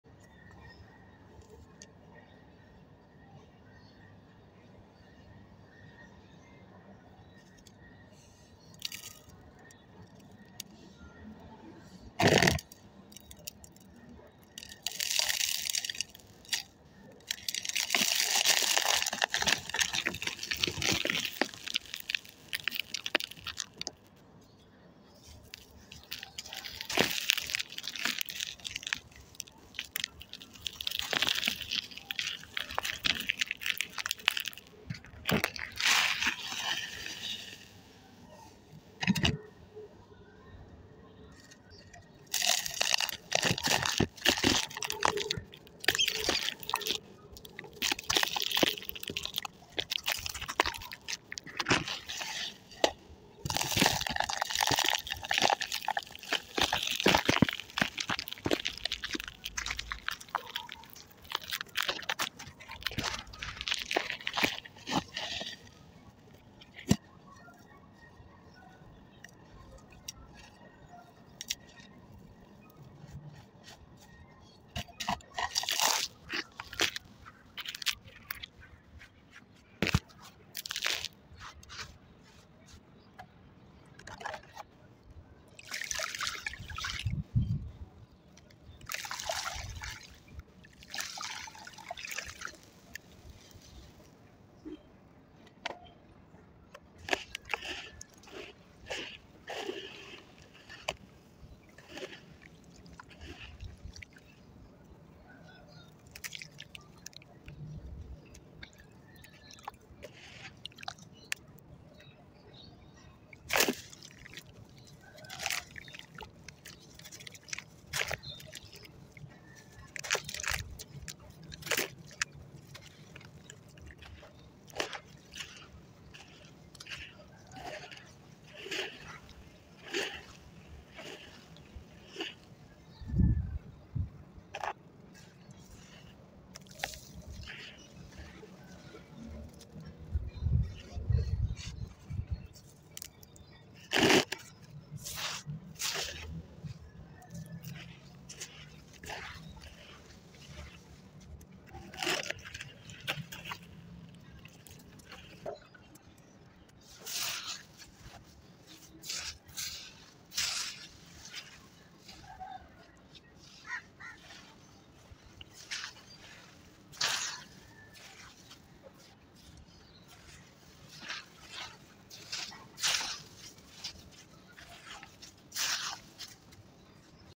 ASMR| Dusty 💨sand cement crumbling| 💦 crumbling, mixing 🤤🤤🤤
Full relaxing n mouthwatering video created by ASMR (autonomous sensory meridian response)  a term used to describe a tingling, static-like, or goosebumps sensation in response to specific triggering audio or visual stimuli. it's give you satisfaction and relaxation.